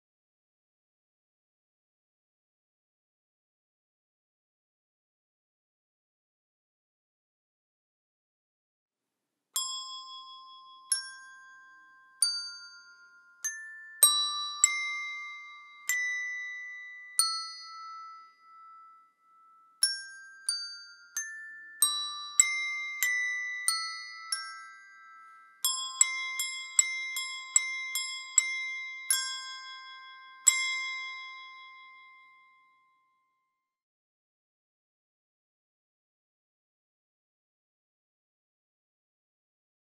Cloches à sonnette
8 cloches musicales de Do à Do
Les cloches ont un son harmonieux (pas strident) et il suffit d'une douce pression sur la sonnette pour obtenir une note cristalline de Do à Do (parfaitement accordée.)

En les disposant "en fleur" avec le Do grave au milieu on peut laisser libre court à son imagination.
Aussi bien pour les enfants que pour les joueurs les plus inventifs !